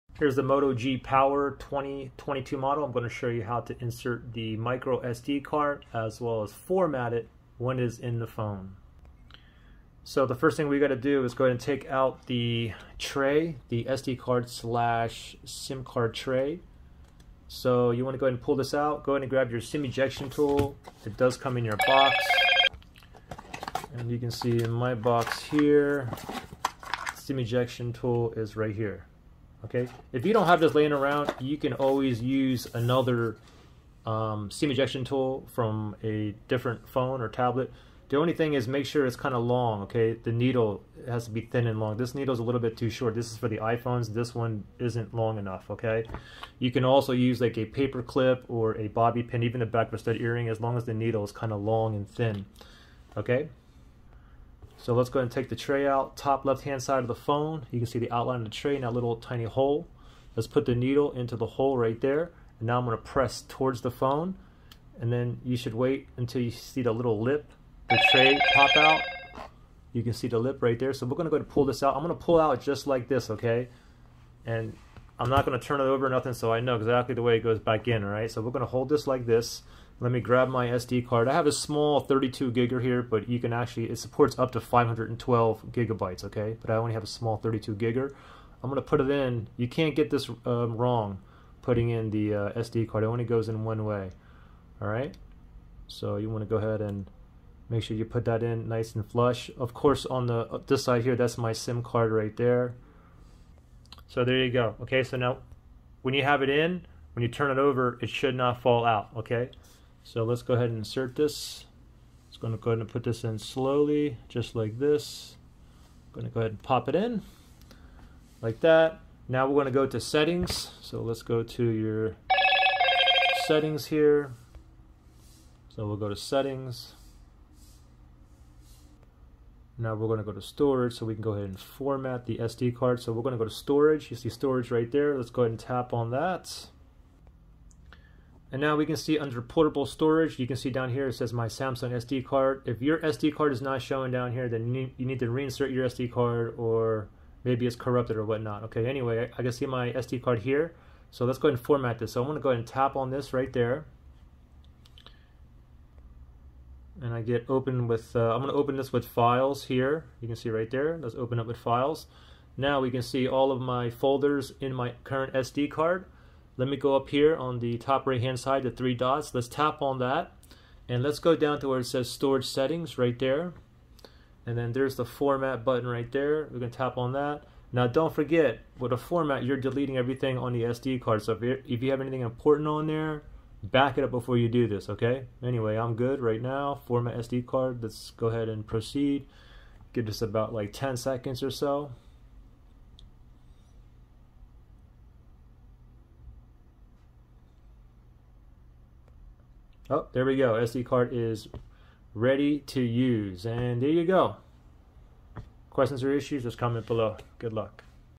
Moto G Power (2022): How to Insert SD Card & Format
I show you how to insert & format a micro SD card in a Moto G Power (2022) smartphone. Hope this helps.

Moto G Power | 2022 | Unlocked | Motorola